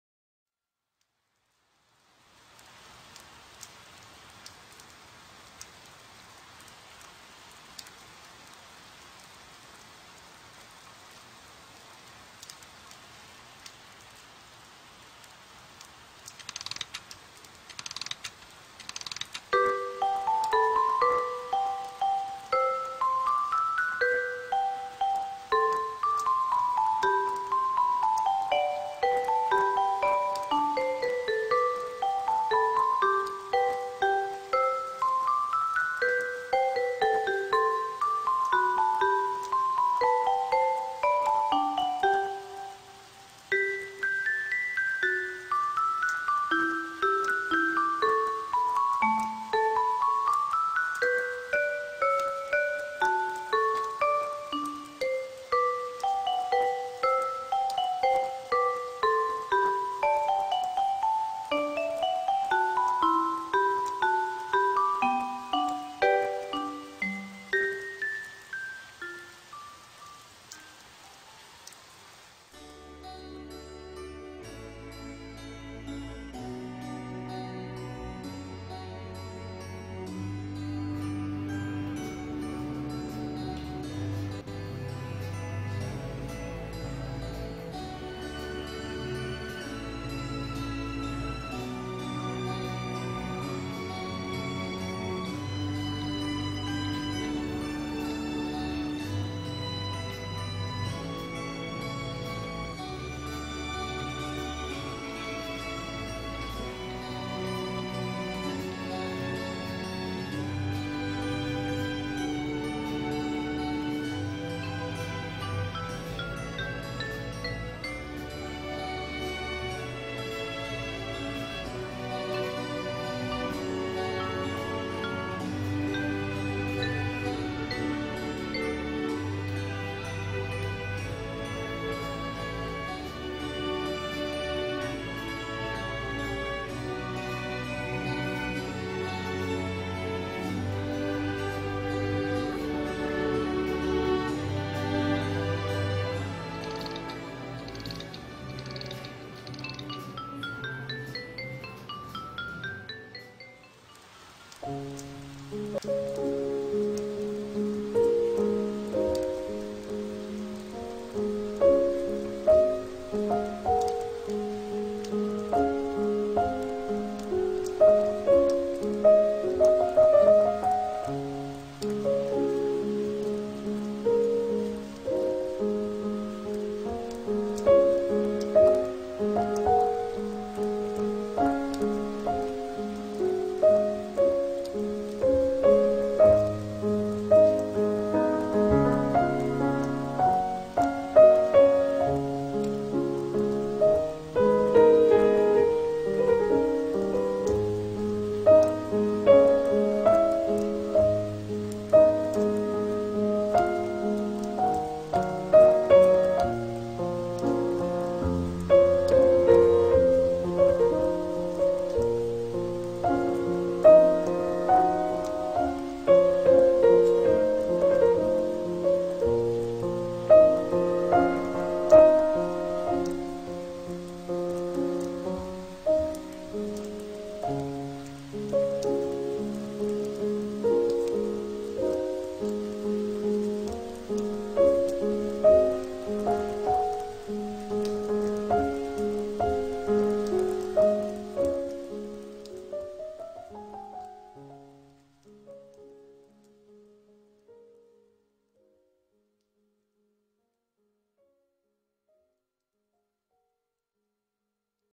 iOrgel
iOrgel is a digitally-reinvented- music box for your iPhone
Supported 24 note music box. The app is growing fast and top ranked in many countries.